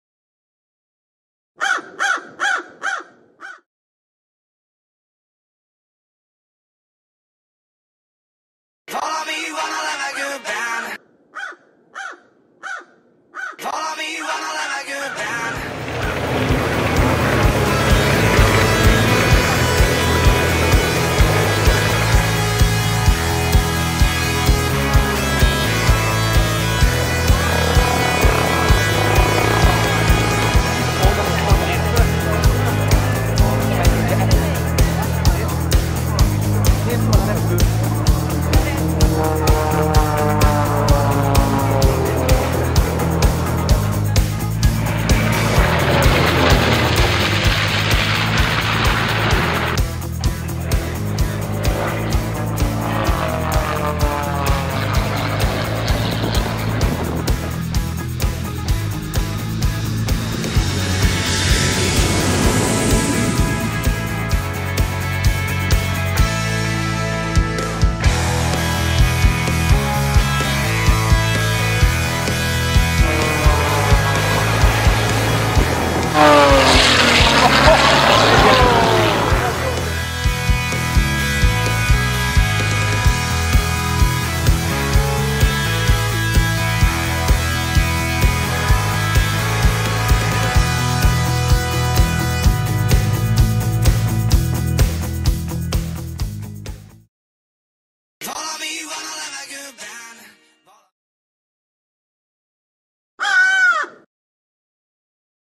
Air Show in Budapest
"Nagy Futam" in Budapest. Besenyei Péter, Veres Zoltán & Flying Bulls in the Air Show. 01 May 2015 in the capital of Hungary.